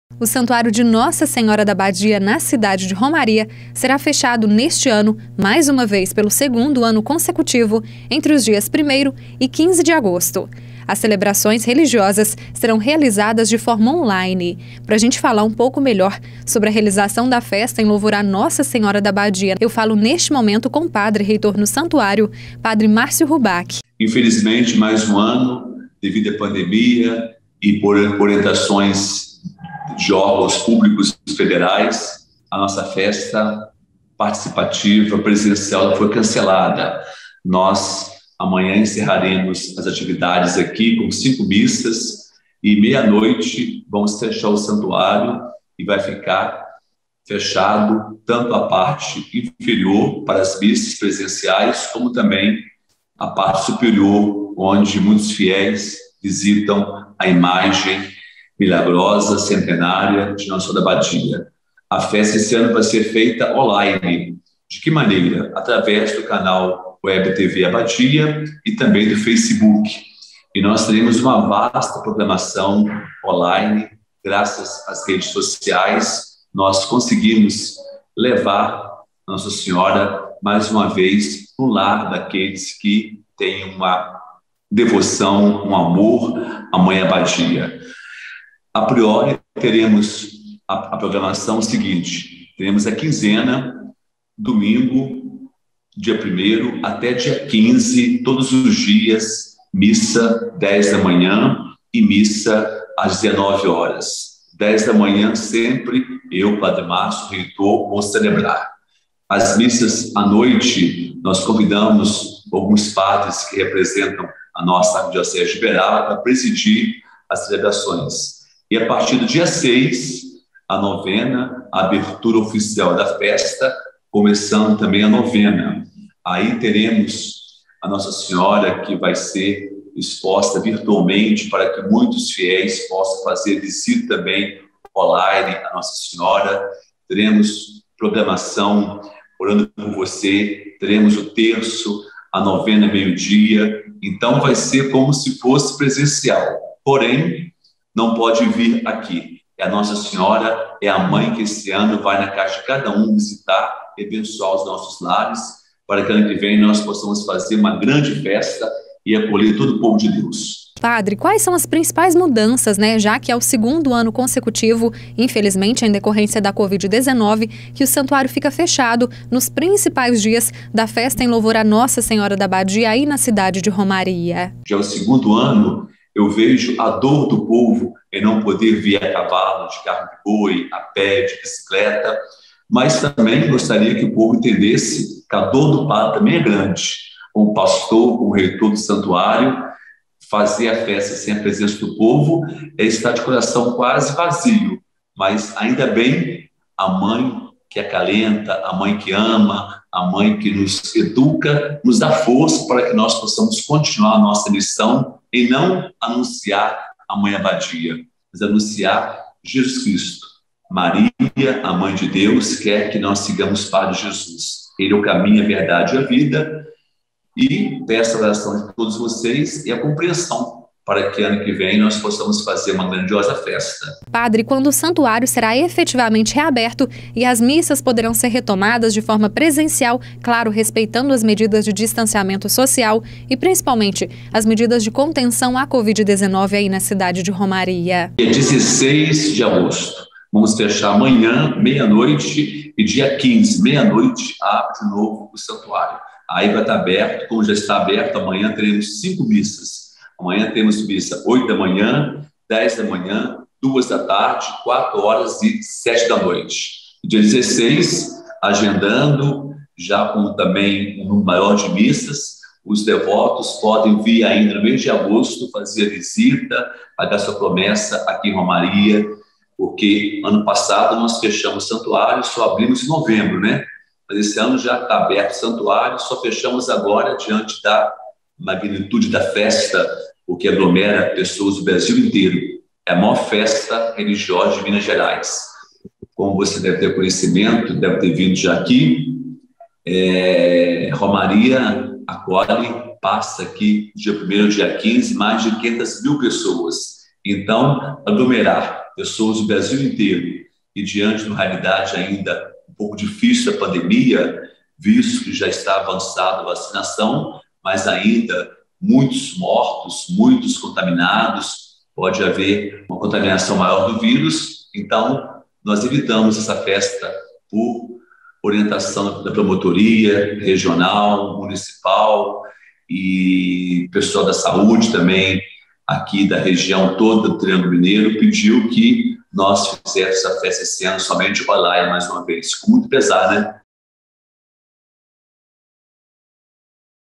Santuário em Romaria será fechado de 1º a 15 de agosto de 2021 e celebrações serão online
O Santuário de Nossa Senhora da Abadia em Romaria estará fechado entre os dias 1º e 15 de agosto deste ano, pelo segundo ano consecutivo como medida preventiva em relação a Covid-19. As celebrações religiosas da Festa de Nossa Senhora da Abadia serão realizadas de forma online, como explica o Padre Márcio Ruback.